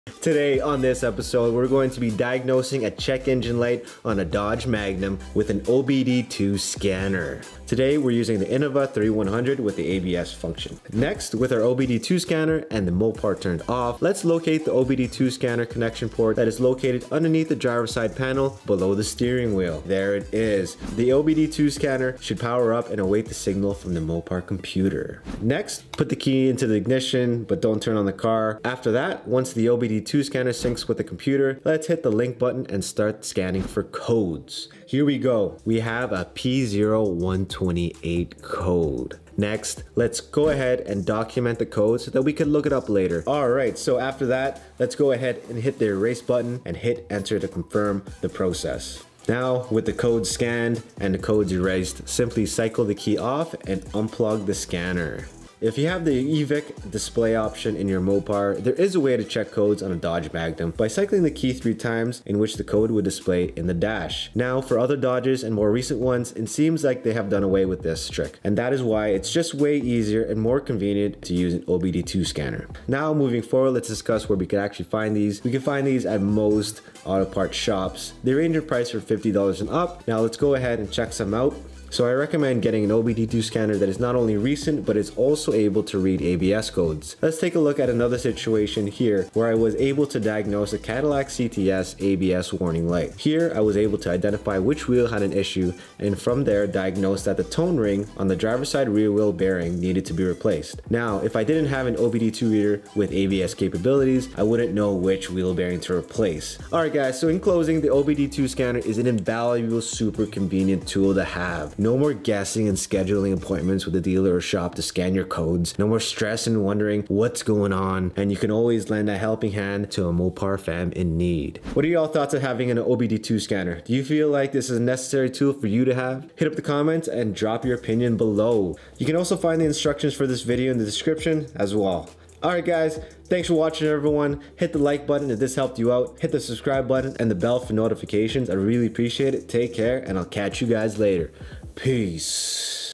how to use obd2 scanner
Check Engine Light with OBD2 Scanner (ODB II) for Dodge Magnum Challenger Charger 300 SRT8 3.5L 3.6L 5.7L 6.1L 6.4L 392 with Fred_Eazy21 !

What's up Mopar fam what's going on? Today on this episode we are going to be diagnosing a check engine light on a Dodge Magnum with an OBD-2 Scanner.

1)OK so First we need an OBD-2 Scanner, if you don't have one call a friend that does or go out and get one, well discuss where to get one, how much they generally cost and what I recommend to get at the end of the video! Today we are using the innova 3100 ABS!

2) Next with our OBD-2 Scanner and the Mopar turned off lets locate the ODB-2 connection port that is located underneath the driver side panel below the steering wheel. The ODB2 scanner should power up and await the signal from the mopar computer.

3) After that lets put the key in and put the mopar in the “On” Position.
 once The OBD2 scanner syncs with the computer let's hit the link button to start scanning for codes.

4)  Next let's document the code so that we can look it up later, this OBD2 reader gives us a short description of what the issue is, which is handy but even the same codes on different vehicles may vary, so it's always a good idea to do some research on the code based on your own mopar model!

From this point on we shall see how long it takes for the check engine light to come back on and then pay attention to the driving conditions in which the code throws back on. Now in some cases they never come back haha, I have had a bunch of times where they don't come back especially with codes pertaining to sensors.

6) Now with the codes scanned and codes erased simply cycle the key off and unplug the scanner.